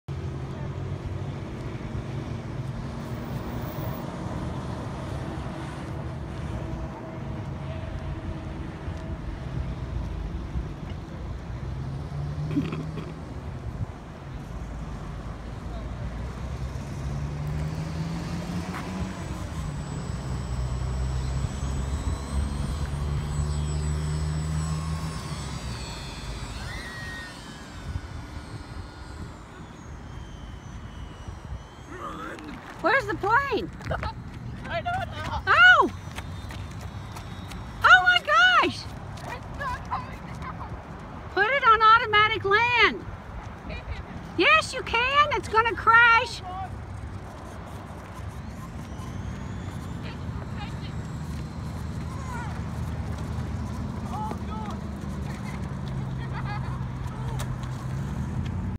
RC plane (ITS GONG TO CRASH)😳😳😳
Tommy's flying the plane Daniel is driving the car and my grandma is holding the camera.